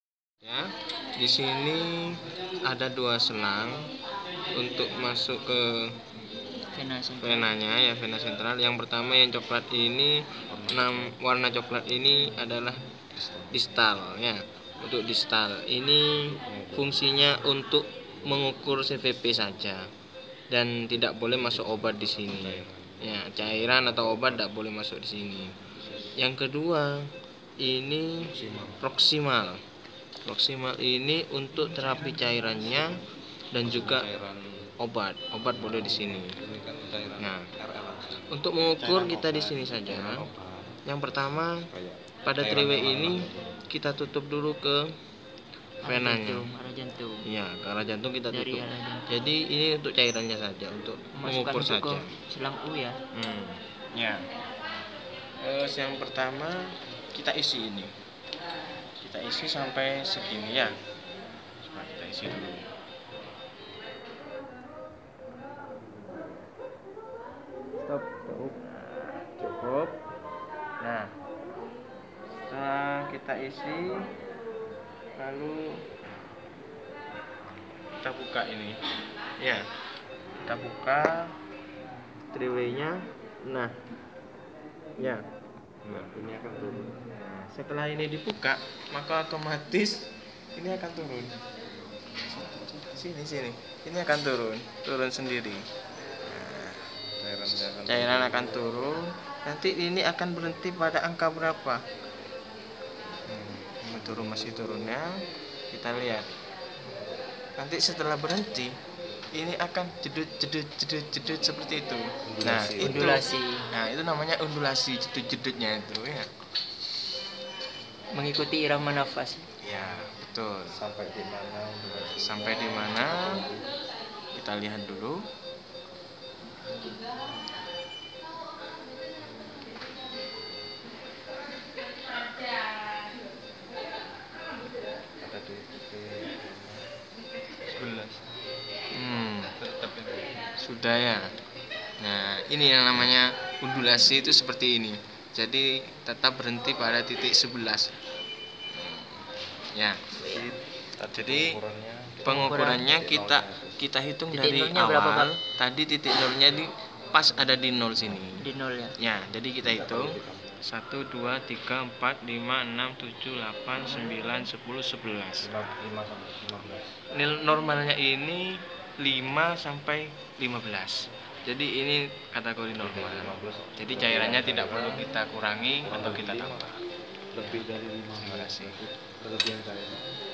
pengukuran central vena pressur CVP lengkap
berikut cara mengukur central venous catheter CVC,lengkap dengan penjelsan.semoga bermanfaat